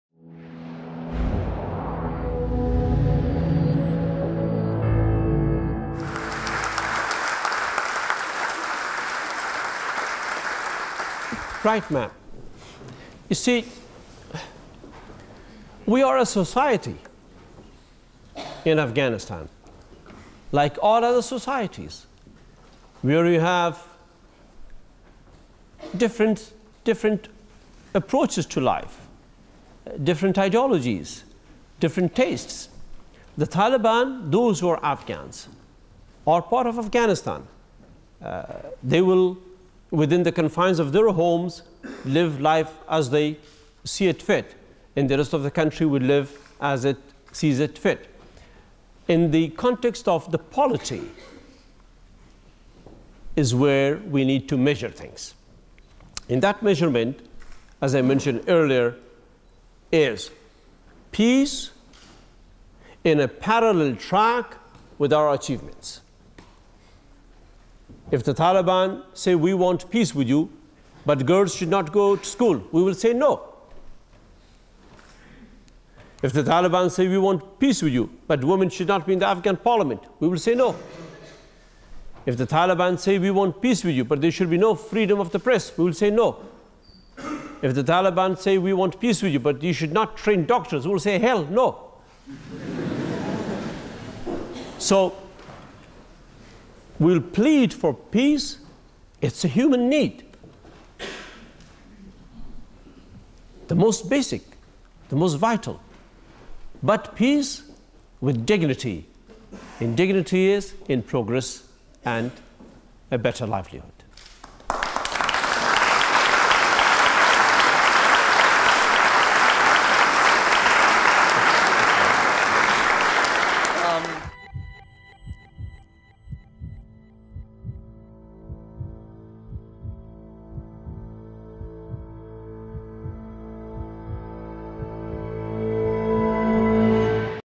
The Taliban | Hamid Karzai | Oxford Union
President of Afghanistan, Hamid Karzai Talks about the peace negotiations with the Taliban.

Hamid Karzai speaks about Taliban peace negotiations. If the conditions of peace prevents progress then there shall be no peace. Peace will be found in dignity and lead to a better livelihood.
Filmed on Friday 1st November 2013

ABOUT HAMID KARZAI:
Hamid Karzai is the 12th and current President of Afghanistan, taking office on 22 December 2001. He became a dominant political figure after the removal of the Taliban regime in late 2001.